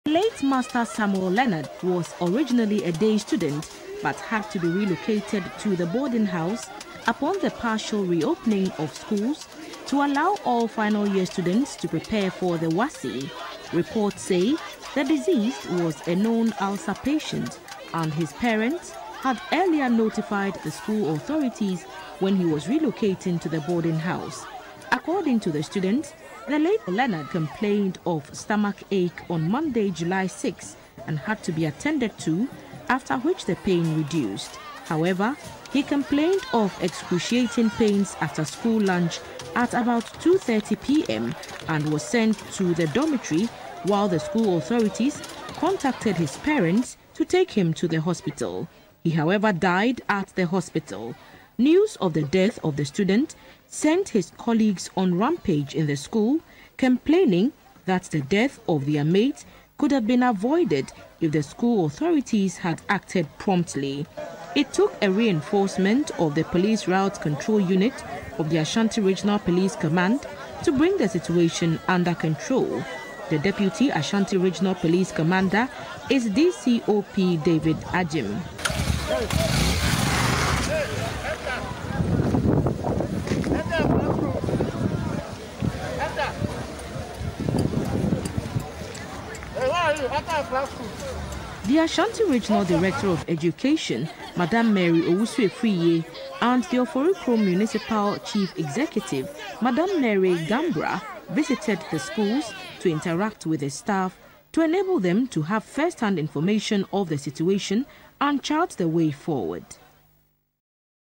Uneasy calm at KNUST following death of student
The Director General of GES has directed the Headmistress of KNUST Senior High School, Felicia Asamoah Danquah to step aside for the Ashanti Regional Director of Education to take over the administration of the school while Management of GES investigate the circumstances leading to the demise of a student which sparked agitation by students.